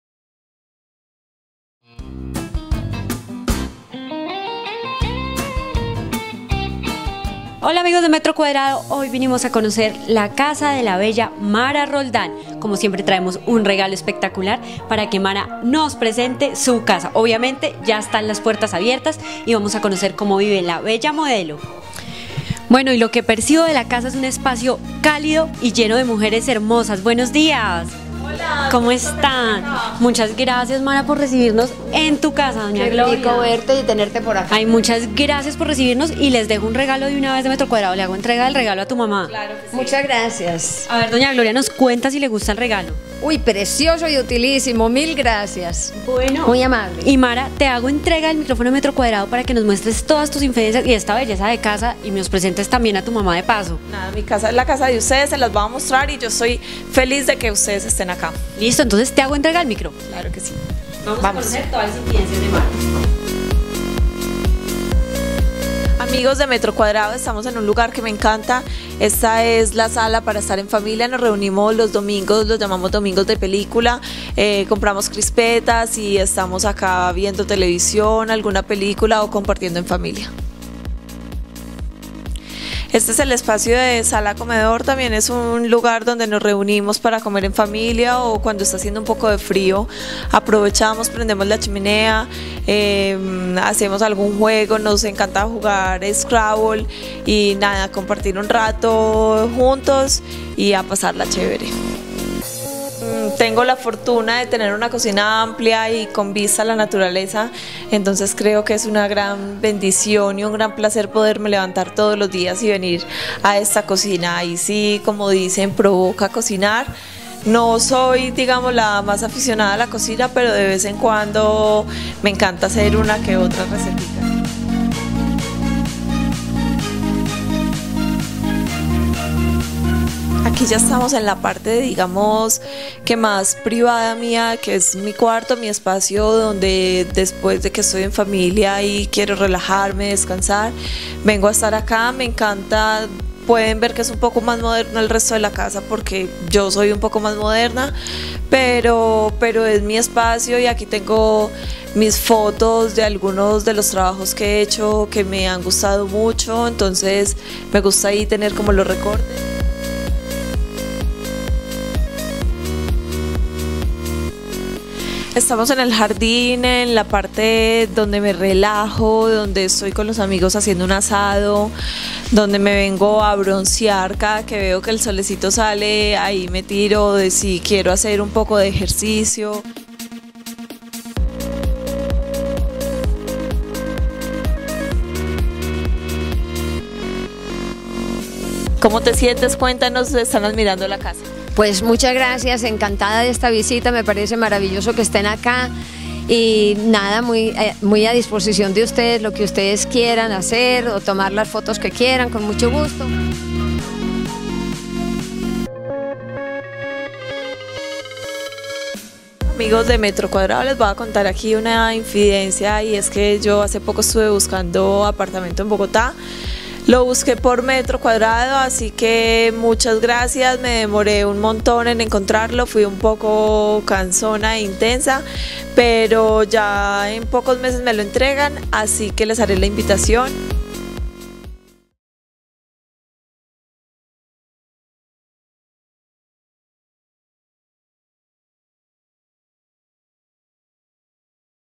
Mara Roldán muestra su casa
La bella y sensual modelo paisa Mara Roldán, dejó entrar a Metrocuadrado a su casa en Medellín. Decoración exquisita, ventanales diseñados para disfrutar de luz natural y 3.500 metros para deleitarse con un  hermoso paisaje hacen parte de la visita que Metrocuadrado realizó en su sección Casas de Famosos.